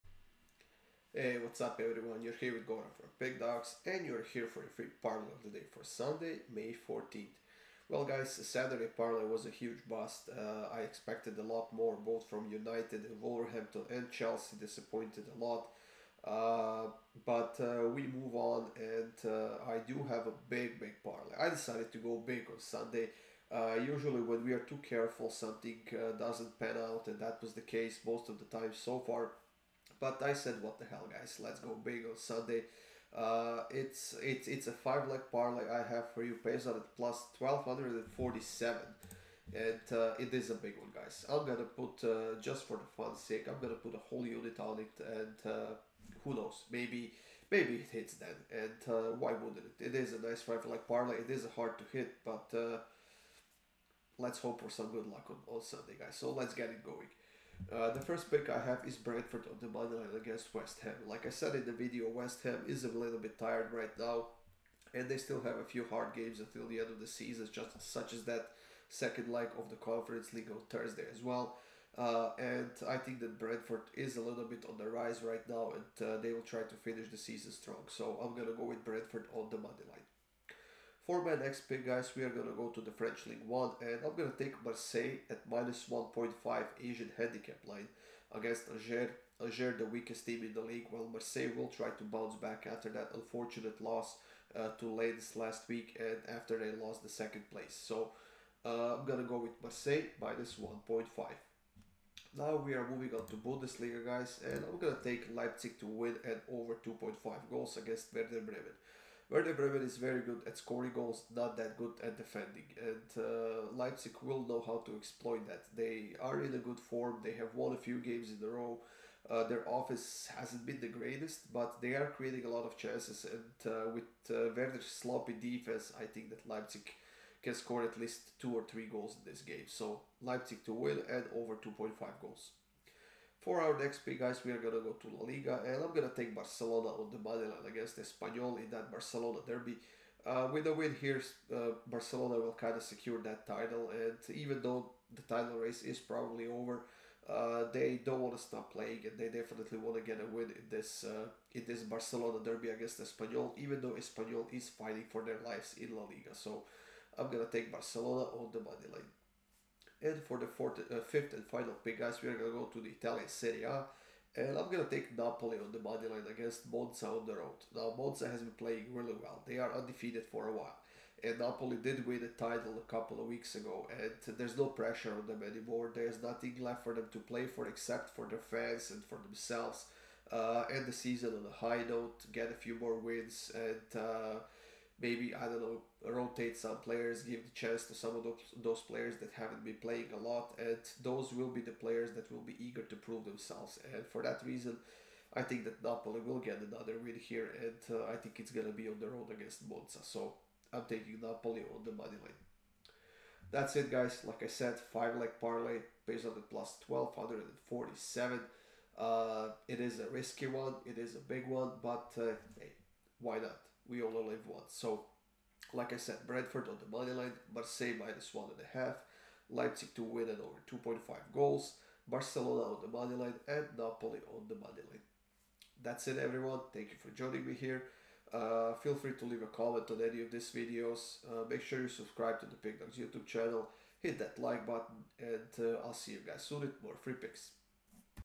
Soccer Parlay of the Day 5/14/23: Goran's Corner Kick | Parlay of the Day Soccer Free Picks
A Football Parlay for Sunday.. Get Goran's Football Pick and Prediction for this game.